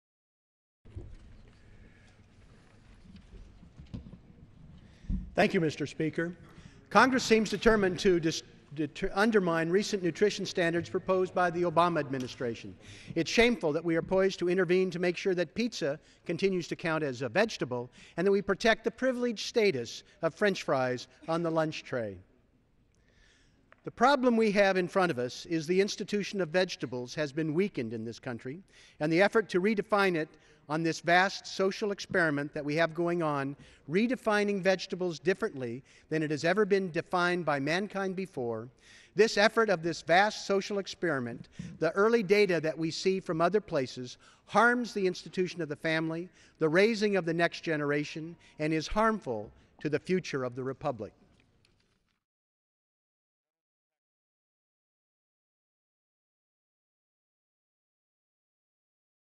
Pizza as a Vegetable?!
Congressman Blumenauer highlights the creeping danger posed by re-defining pizza and french fries as "vegetables" in our school lunch program. The traditional definition of vegetables can help lead us to a future where we are happier, healthier, and big agri-business lobbyists aren't changing the meanings of our words.